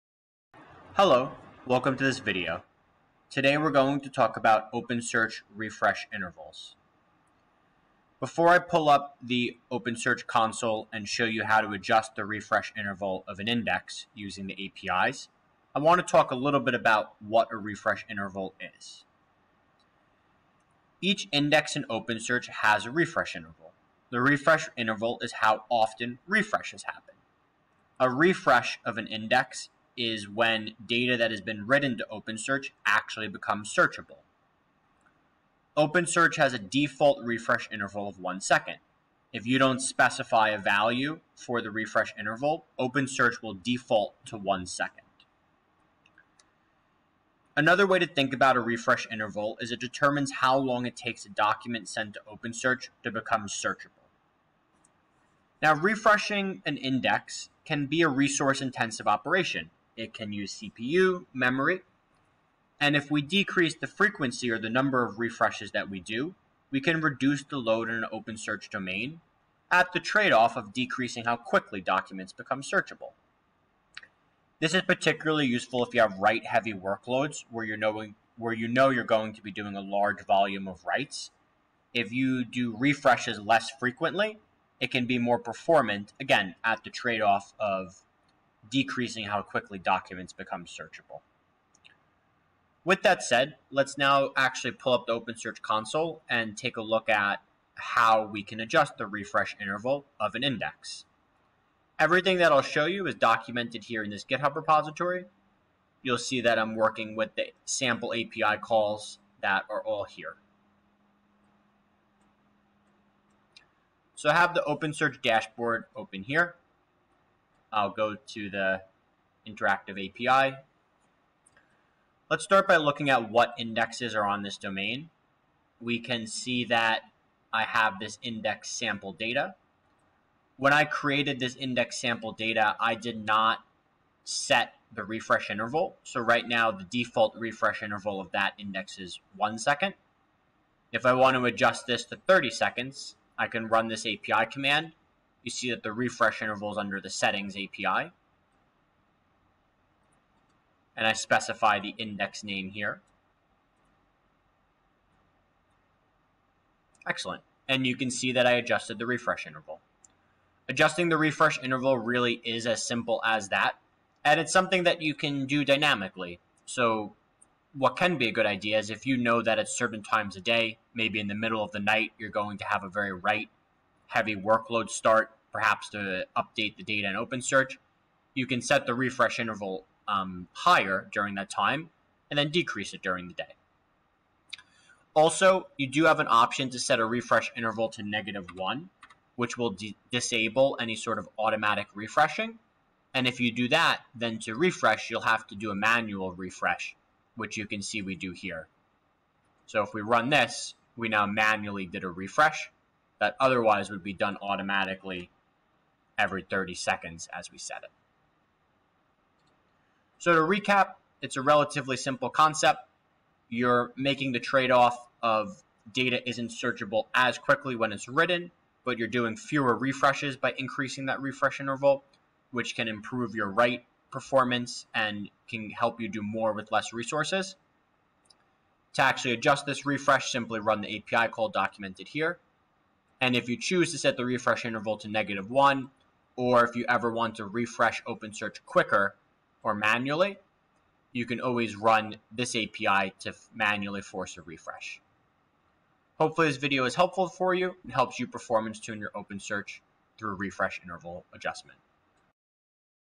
OpenSearch - How to change the refresh interval of an index
Learn how to change the refresh interval of an index in an Amazon OpenSearch domain.

An index refresh interval specifies how frequently an index refreshes. Refreshing an index makes data that has been written to OpenSearch search-able.

Refreshing an index can be a resource intensive operation, decreasing the frequency of refreshes can reduce the load on an OpenSearch domain at the cost of decreasing how quickly documents become search-able.

Adjusting the refresh interval of an index can help you achieve better performance esp. for write heavy workloads.

GitHub repository from the video with detail instructions including the API commands